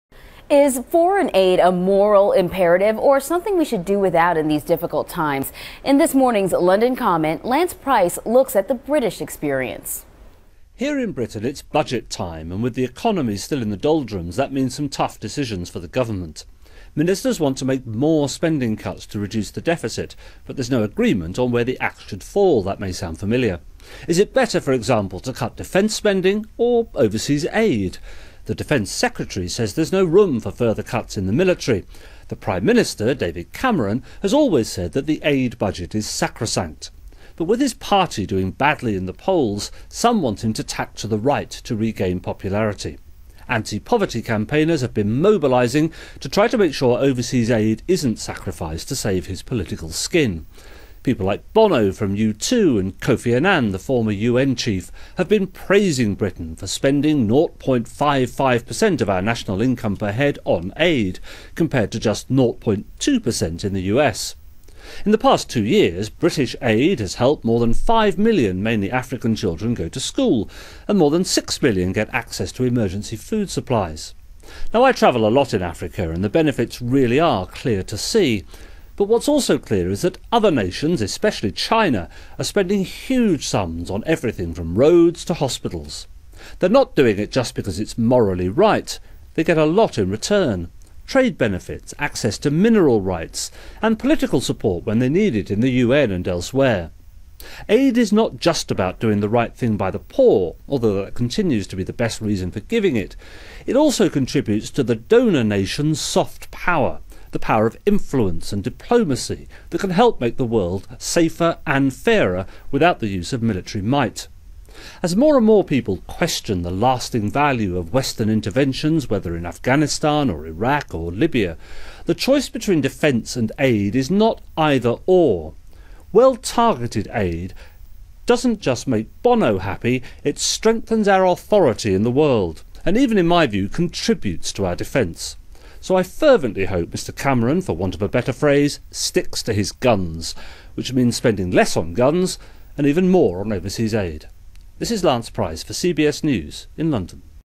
Defence and Aid
Is foreign aid a moral imperative, or something we should do without in these difficult times?  UTTM contributor Lance Price takes a look at the British experience.